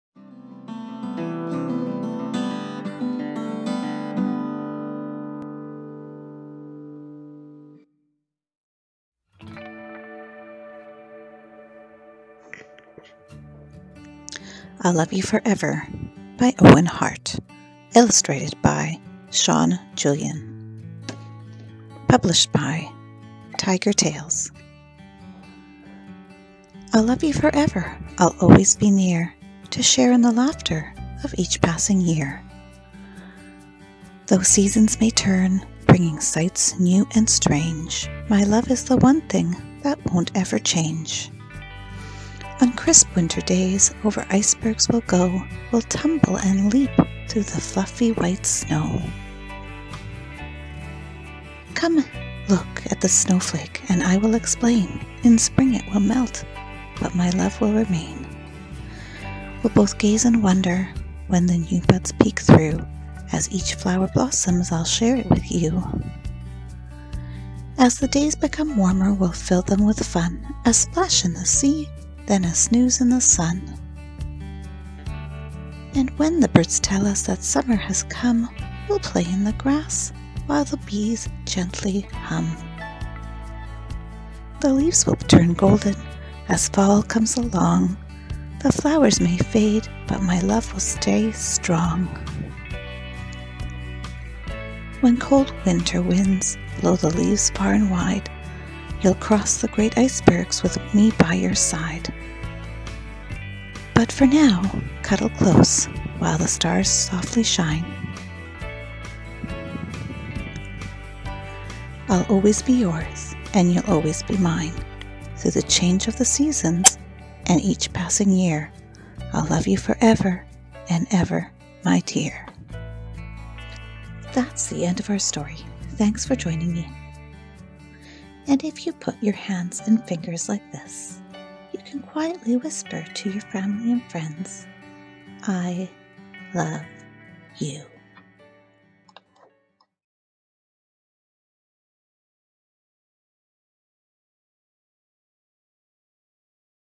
Miss Odette Reads I'll Love You Forever
Miss Odette reads to you! Join her as she tells the story of I'll Love You Forever written by Owen Hart and illustrated by Sean Julian.